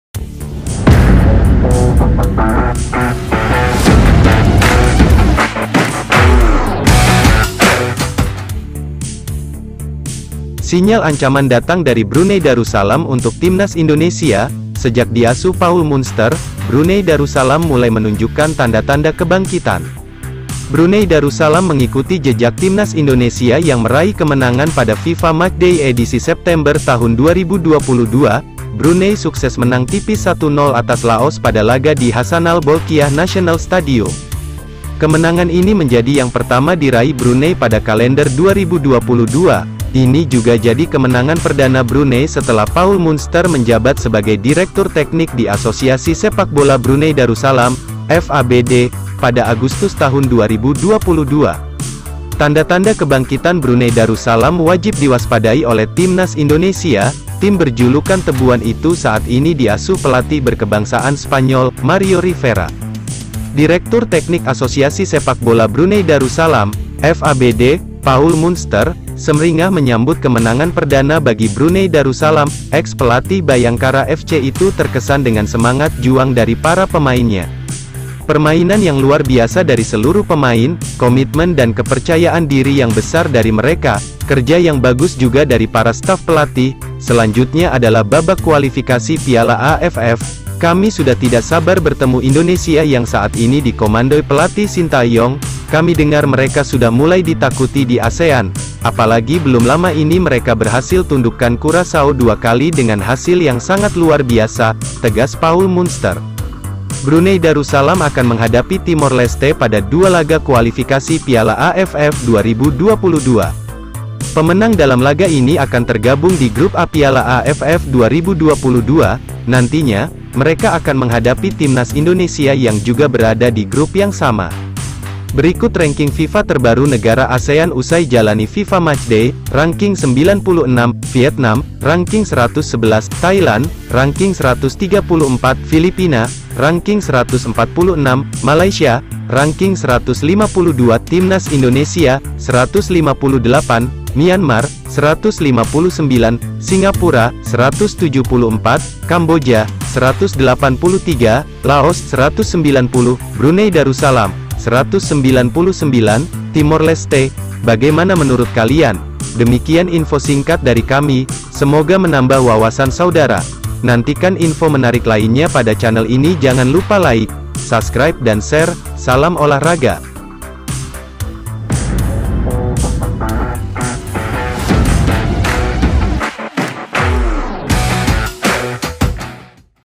Paul Munster Berikan Sinyal Bahaya Tantang Timnas Indonesia Di Piala AFF
BERITA SEPAKBOLA TERBARU DAN TER UPDATE DARI LINGKARAN TIMNAS.
Berita Sepakbola Hari kamis, 29 September 2022